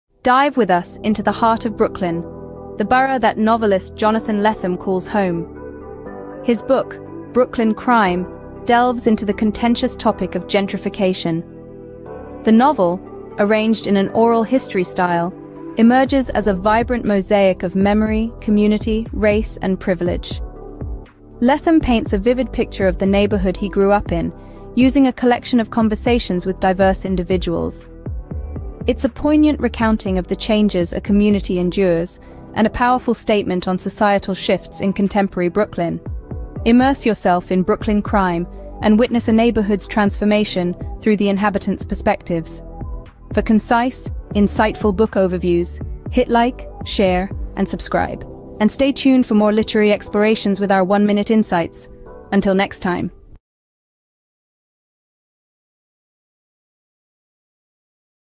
1 Minute Summaries: Brooklyn Crime Novel by Jonathan Lethem
Delve into the narrative landscape of Brooklyn as portrayed in Jonathan Lethem's 'Brooklyn Crime Novel.' Discover a tale of gentrification, memory, community, race, and privilege told through an oral history style.